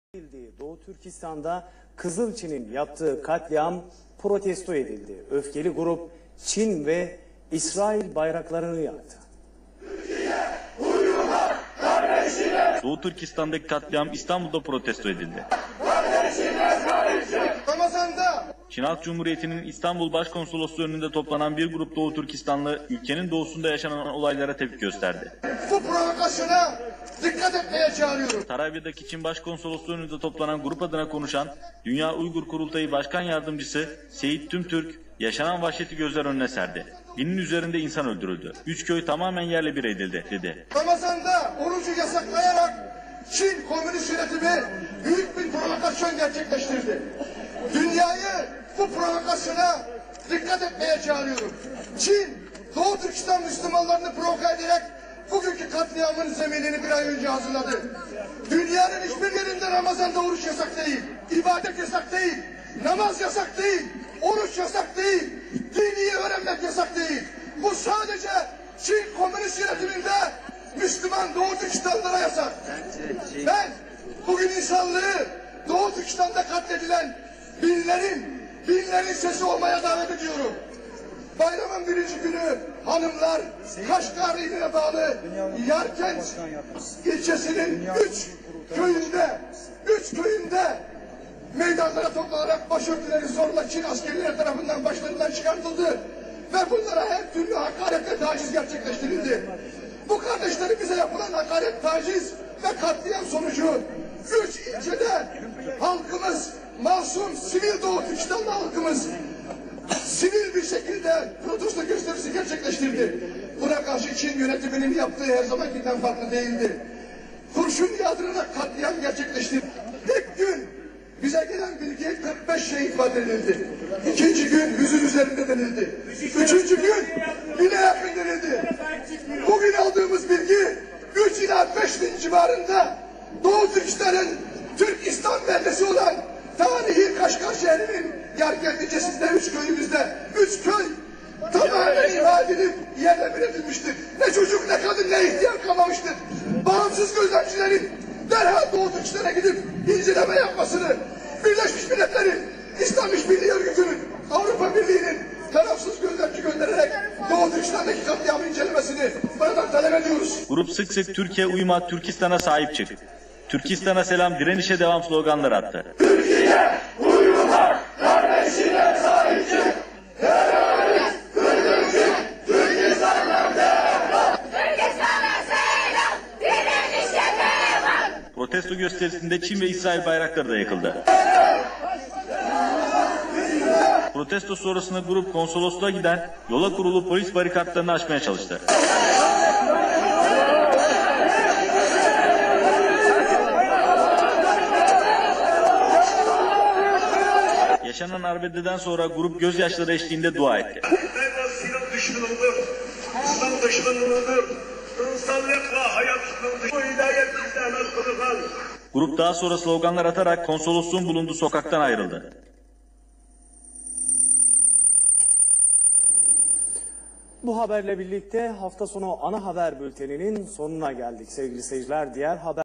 "Çin Doğu Türkistan da katliam yapiyor"Seyid Tumturk,Cin zulmu,Cin mallara boykot
3 agustos 2014 Tv5
 Çin Doğu Türkistan da katliam yapti.Yargenc kentinde uc koy yerle bir edildi.Muslumanlar islami ozgurce yasayamiyor.

Ama bizim sozde liderlerimiz den ciddi bir kinama yok.Neden Cin e de " one minute " denilmiyor ? '' Bizim Kibris diye bir sorunumuz yok " diyen AKP den ne beklenir !? Binlerce Kurd kardesimizin katili apoyu serbest birakmaya hazirlanan AKP den ne beklenir !? Allah in verdigi suyu ozel sirketlere gelir kaynagi yapan AKP den ne beklenir !?